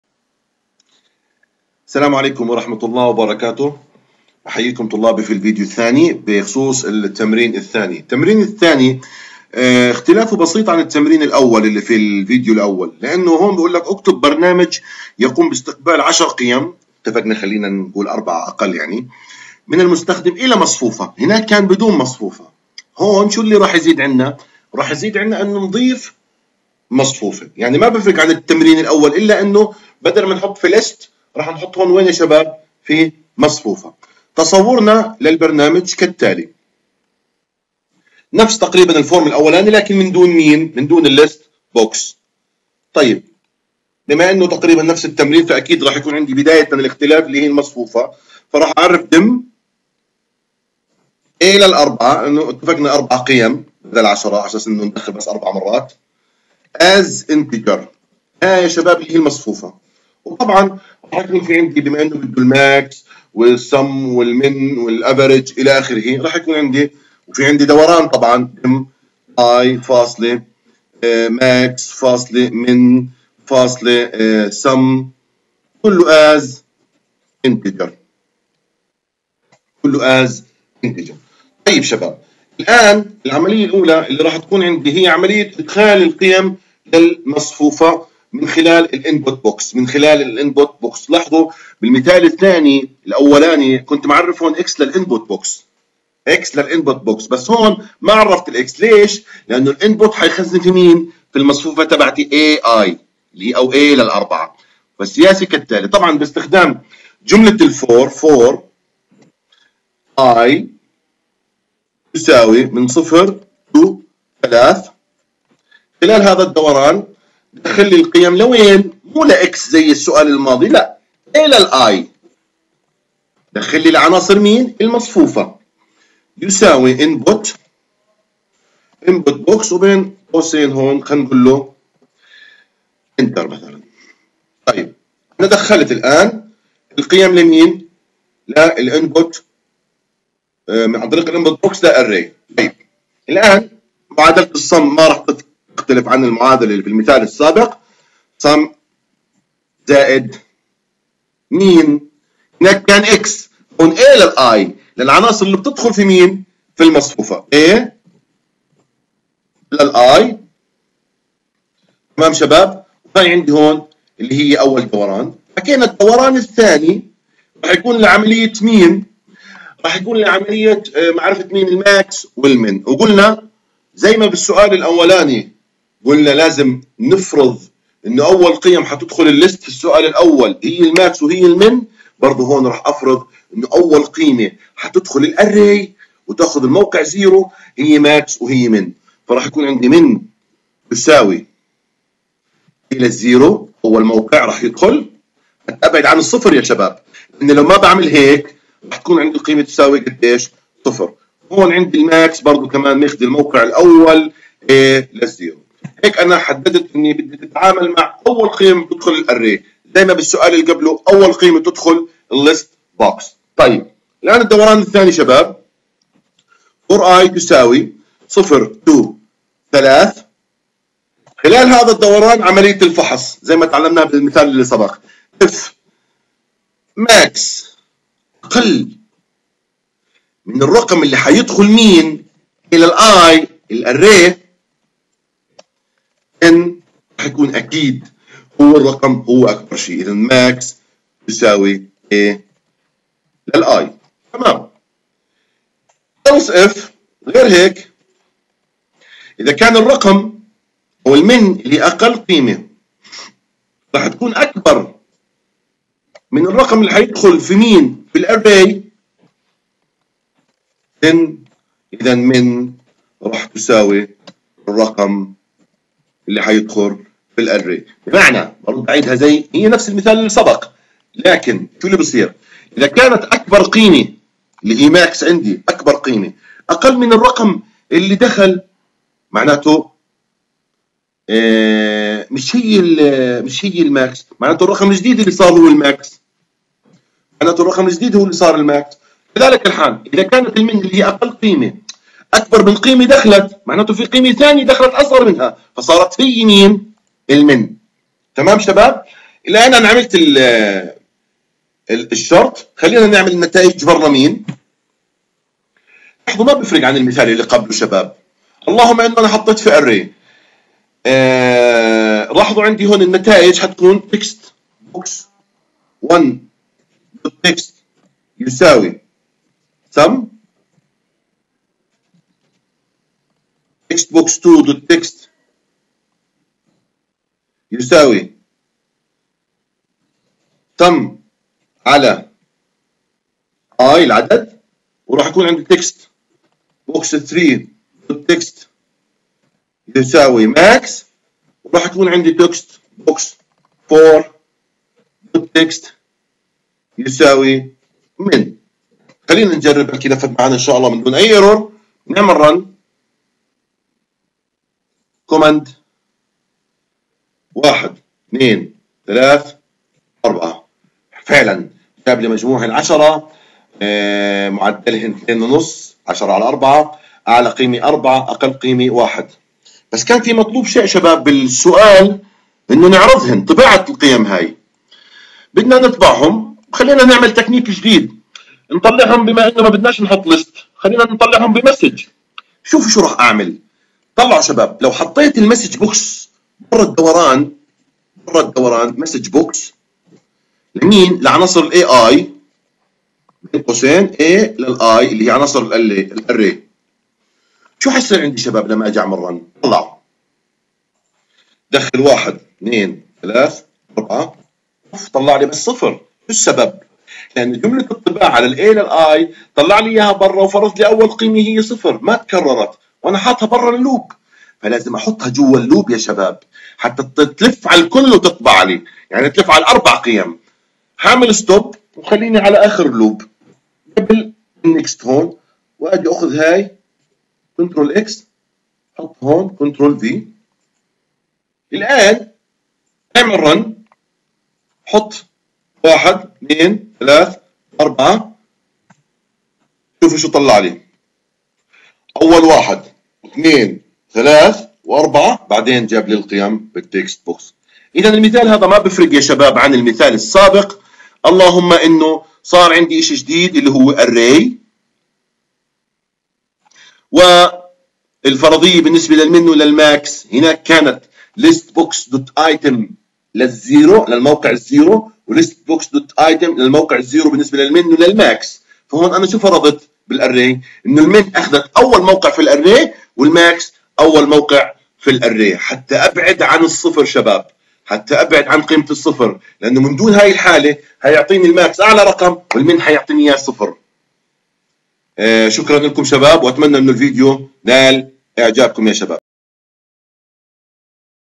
برنامج يقوم باستقبال 10 قيم من المستخدم الى مصفوفة ثم طباعة القيم المدخلة وحاصل جمع هذا القيم وأكبر
اكتب برنامج يقوم باستقبال 10 قيم من المستخدم الى مصفوفة ثم
• طباعة القيم المدخلة و
• حاصل جمع هذا القيم
• أكبر رقم بالعناصر
• أصغر رقم
• متوسط العناصر